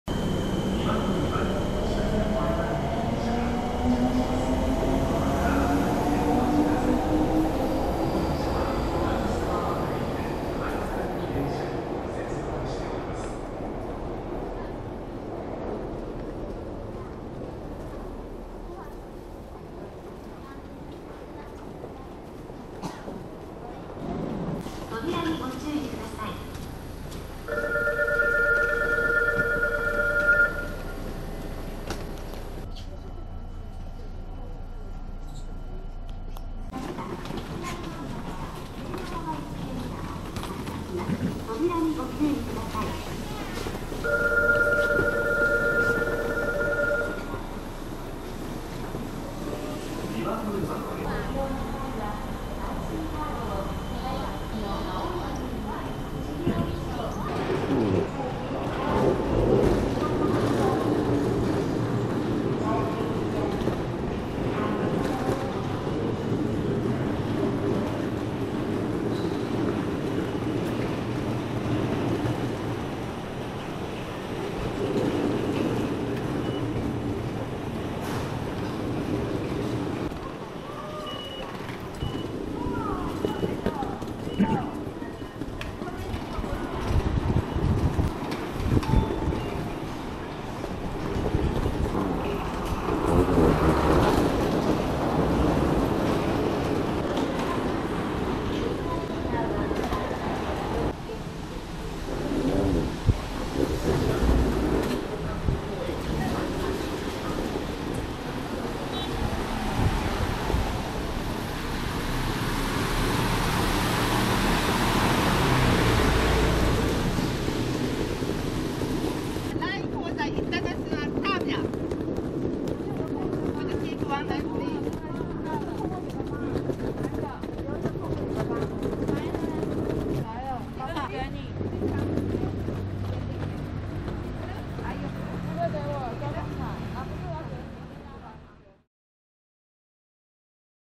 【福岡地下鐵】空港線機場站 Hakata (Japan)
福岡機場車站，位於日本福岡縣福岡市博多區大字下臼井的地下鐵車站，隸屬於福岡市交通局。 福岡機場車站在福岡機場的地下。 不過，該站只連接至福岡機場的國內線航廈，並不連接國際線航廈。